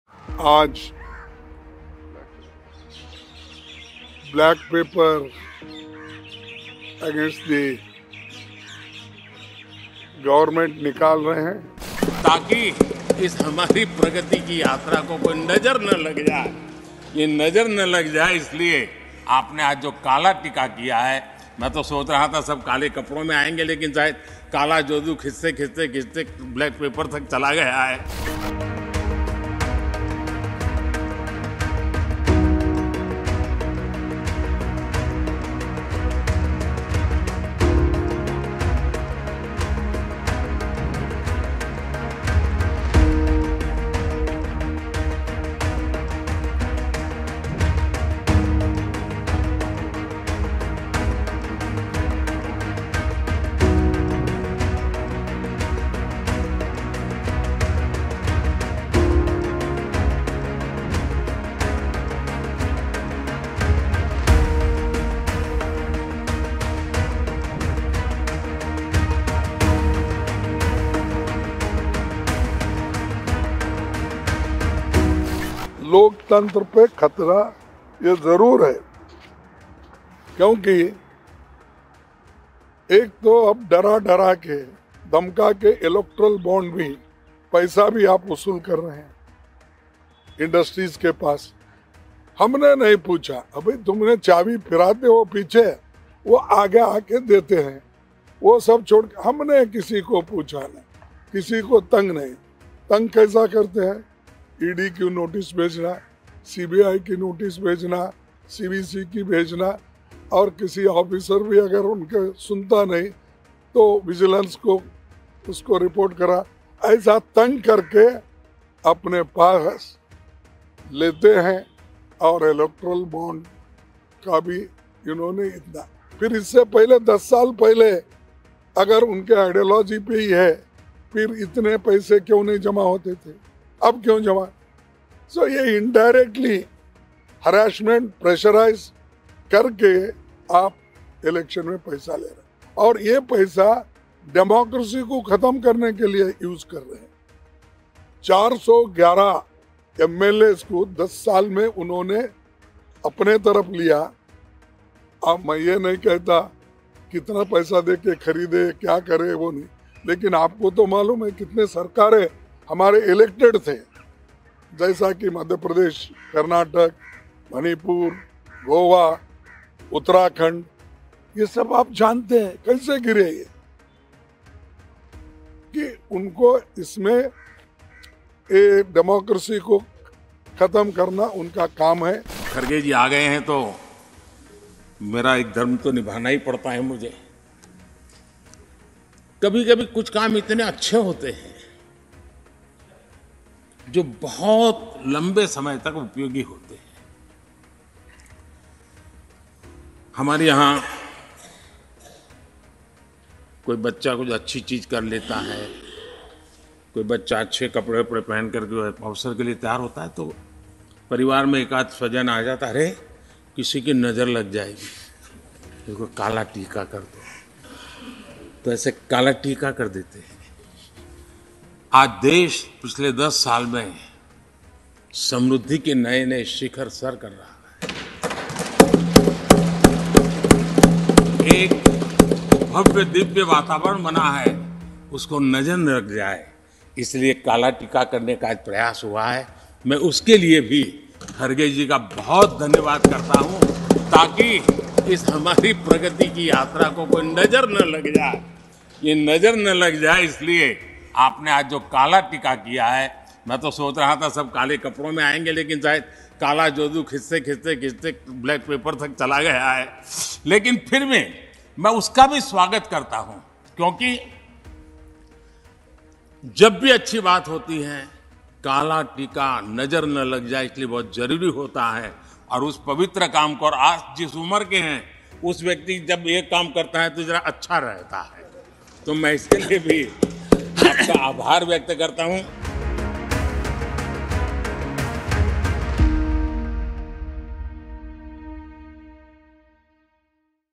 Kala Tika For Our Good Work: PM Modi’s Dig At Congress’ ‘Black Paper’ |Centre Releases ‘White Paper’
Congress President Mallikarjun Kharge Releases 'Black Paper' Critiquing Modi Government's Failures Before White Paper on Economy Presentation"

Mallikarjun Kharge Warns of Democracy Threat, Highlights BJP's Role in Toppling Congress Governments and Ignoring Unemployment Issue
In Parliament, Finance Minister Nirmala Sitharaman Introduced Interim Budget 2024-25, and announced White Paper to Analyze Pre-2014 Era and Current Situation for Learning from Past Mismanagement.